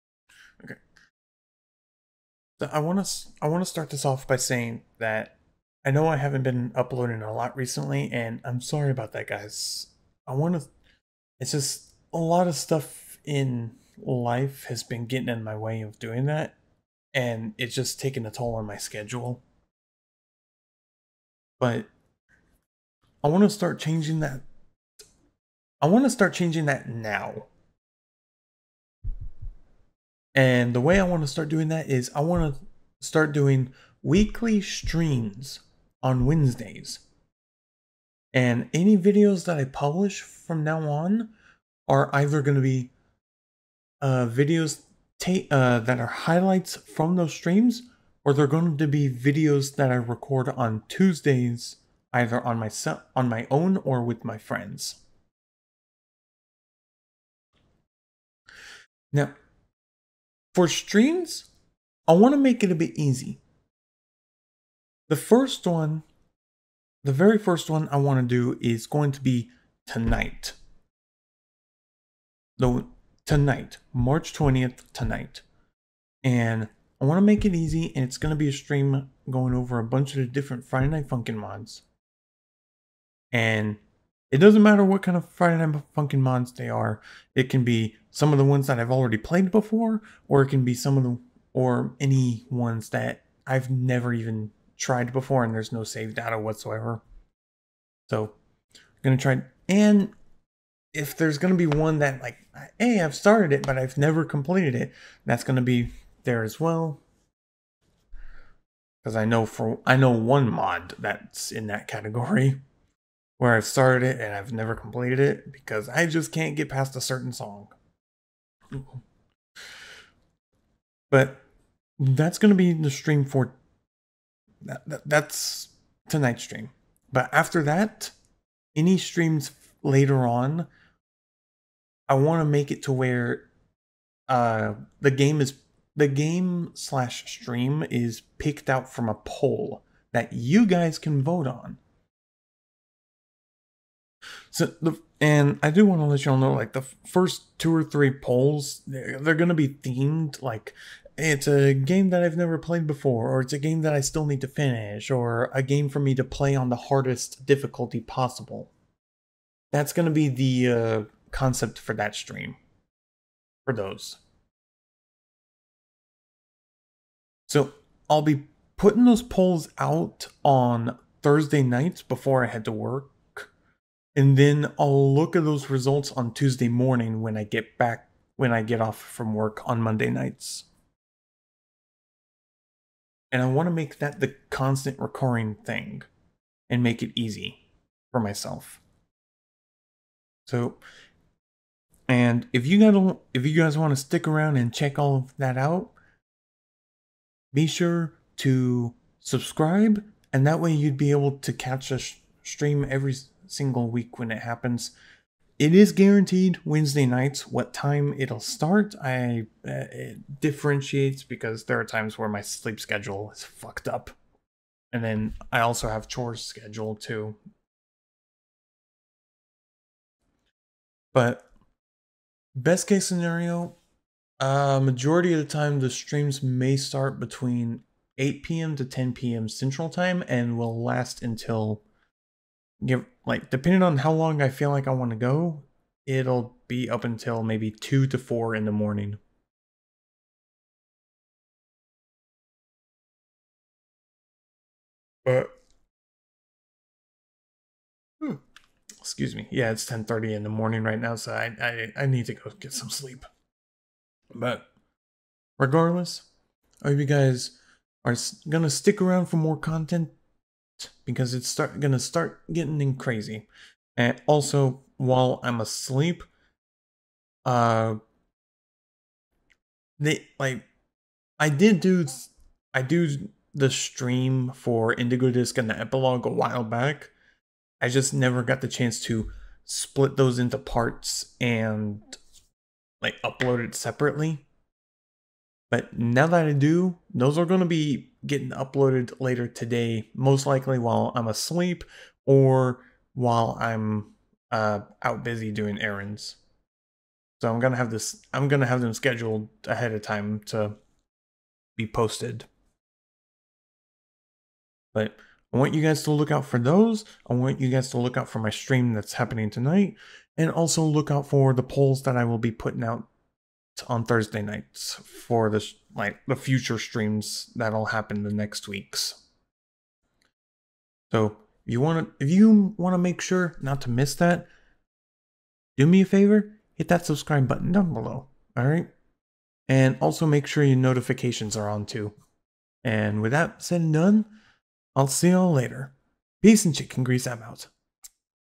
Channel Update March 2024
I'm still alive and kicking. I'm here giving you guys an update on what's going to happen with the channel from here on out. This includes all stuff regarding videos, streams, and community posts.